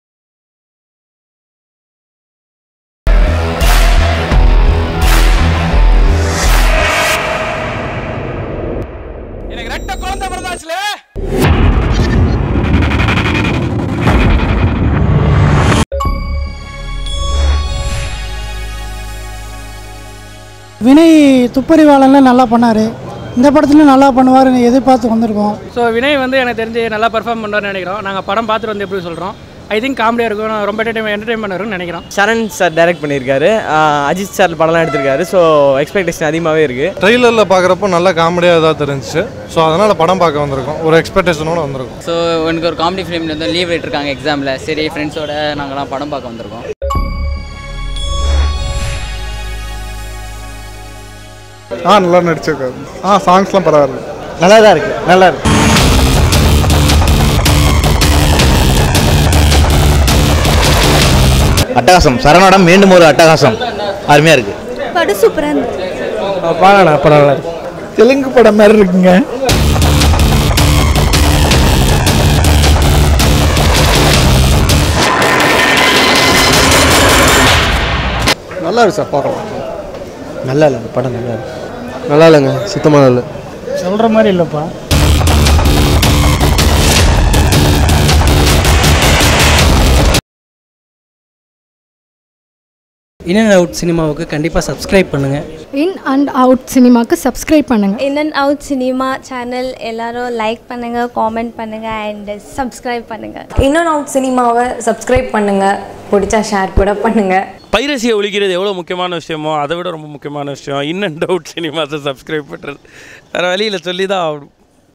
Aayirathil Iruvar Movie Review | Expectation (Vs) Audience Review
Expectation (Vs) Audience Review Of Aayirathil Iruvar

Aayirathil Iruvar is an upcoming Tamil film written, directed and produced by Saran. The film features Vinay in dual lead roles and three newcomers, Samuthrika, Swasthika and Kesha Khambhati, as the female leads, while the director's regular composer Bharathwaj scores the music

Directed by  Saran
Produced by  Saran
Written by  Saran
Starring  Vinay , Samuthrika , Swasthika and Kesha Khambhati
Cinematography  Krishna Ramanan
Edited by  Kevin
Production
company  Saran Movie Factory